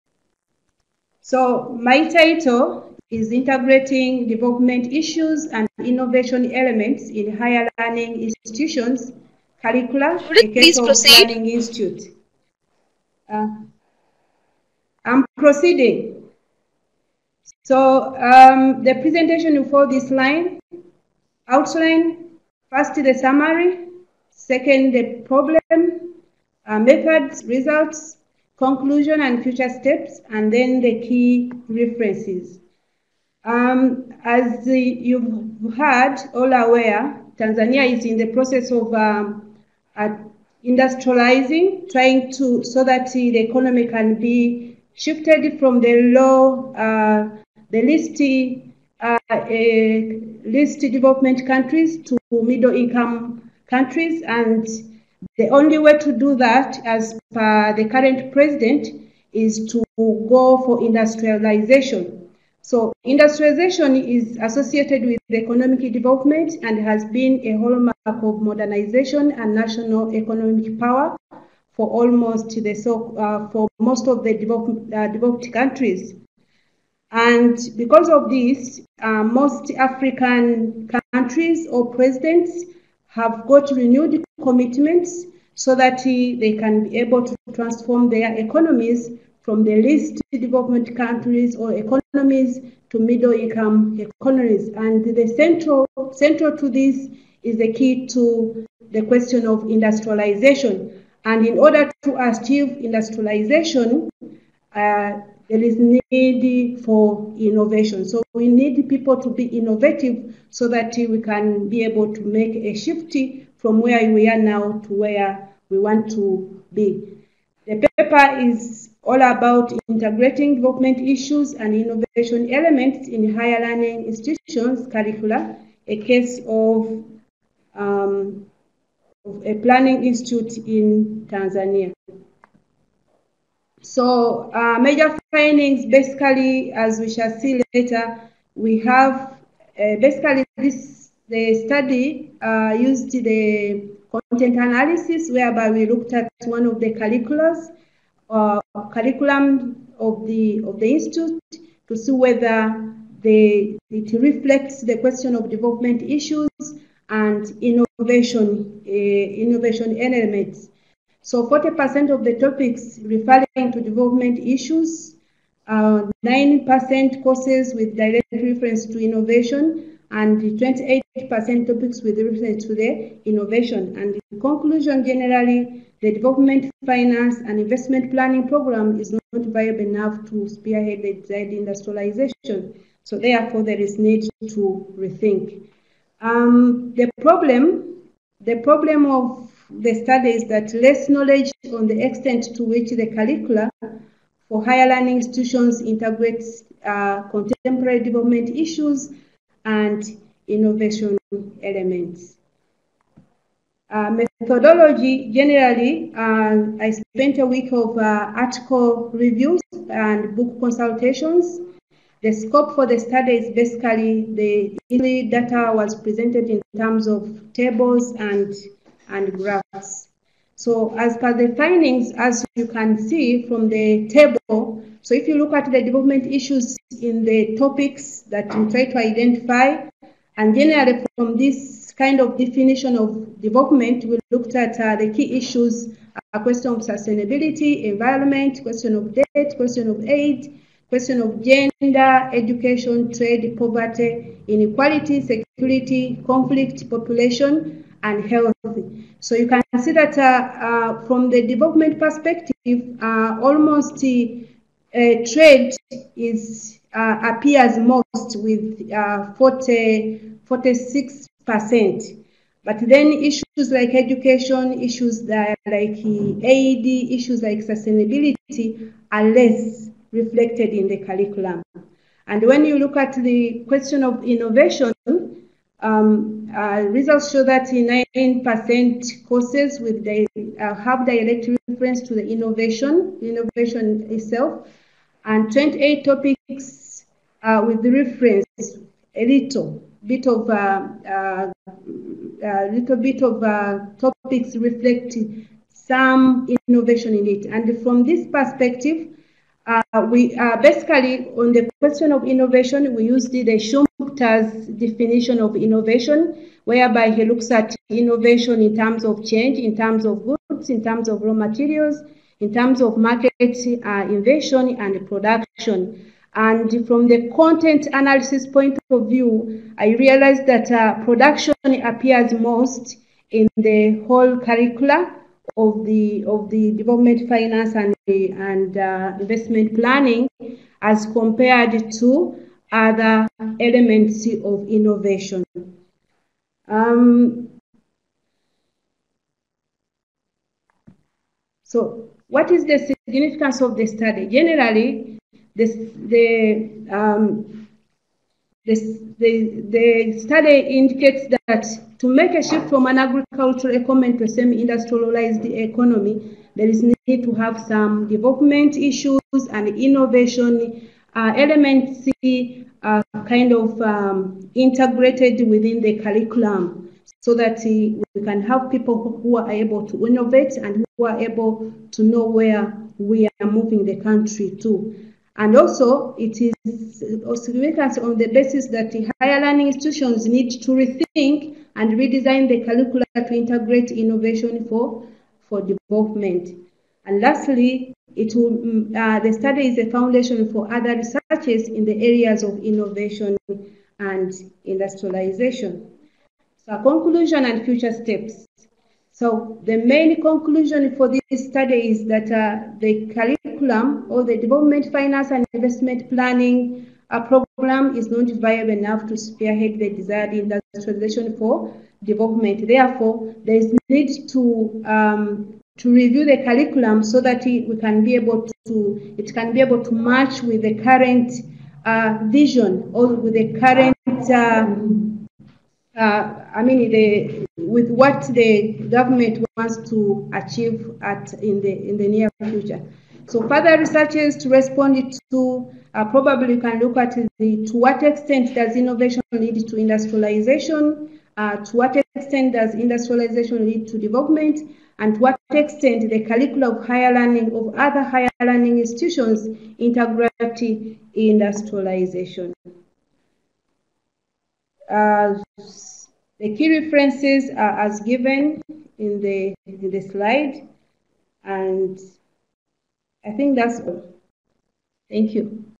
Mrs. Judith Namabira, Tanzania (United Republic of)
An presentation in the 3rd International e-Conference 2016, held on 21st Nov 2016 to 24th Nov 2016 on the topic “Integrating Development issues and Innovation elements in Higher Learning Institutions’ Curricula. A case of a Planning Institute in Tanzania” – Management